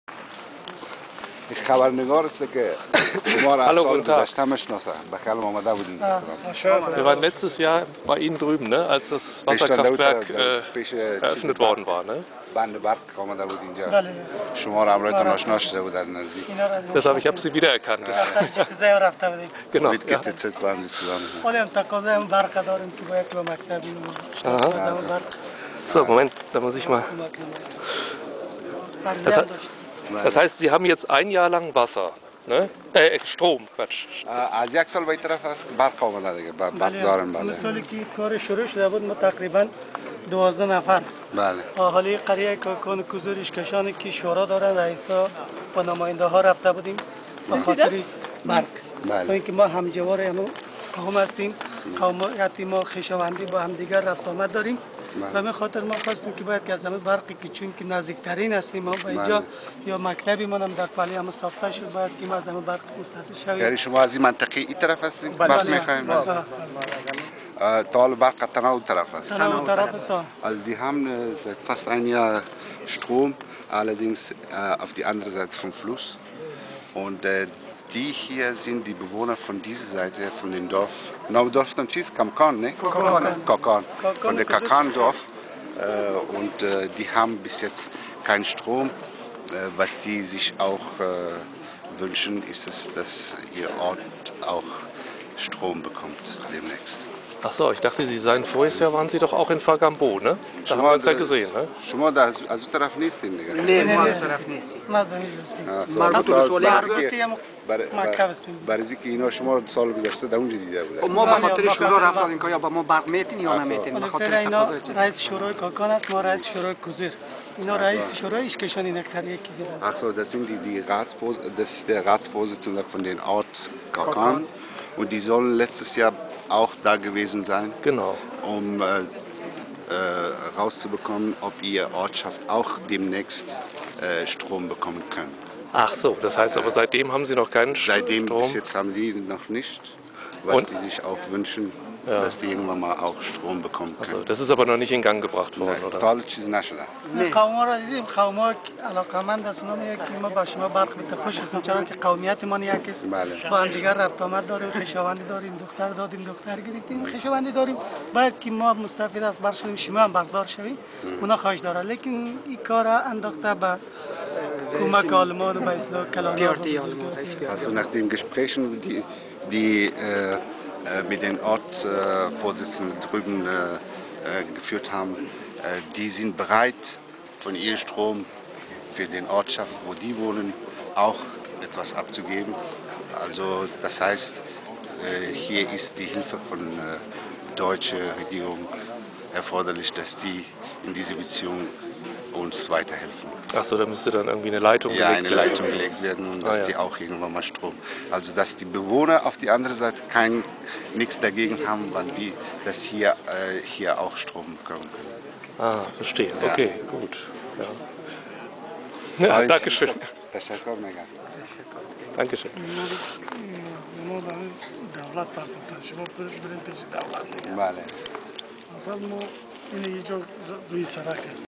Wiedersehen am Kokcha-Fluss (30. März 2011)
Nichtmal ein Jahr nach meiner ersten Journalistenreise kam ich auf meiner zweiten Afghanistan-Reise als Hauptstadtkorrespondent wieder auch in die Provinz Badkhshan. Diesmal im Pressetross des damaligen Entwicklungsministers Dirk Niebel (FDP). Diesmal auch vollständig unter dem Schutz der Bundeswehr, die immer mit dabei war. Wir kamen wieder dorthin, wo das Dorf Fargambow war: Nur auf der anderen Seite des breiten Flusses Kokcha. Hier, im Dorf Kakan gegenüber, erkannte ich einige der Männer, die ich ein Jahr vorher gesehen hatte. Einer zumindest zeigte auch bei mir ein Wiedererkennen. Als ich sie dann fragte, ob sie aus Fargambow kommen, sagte sie "Nein". Ich dachte, ich hätte mich geirrt. Dann allerdings erklärte mir der Übersetzer, dass tatsächlich die Mitglieder der Shura, des Ältestenrates des Dorfes damals bei dem Termin in Fargambow gewesen waren. Sie hatten darum bitten wollen, dass auch sie Strom haben wollten. Bis zu dieser zweiten Begegnung hatten sie dann aber kein Wasserkraftwerk und damit Strom bekommen.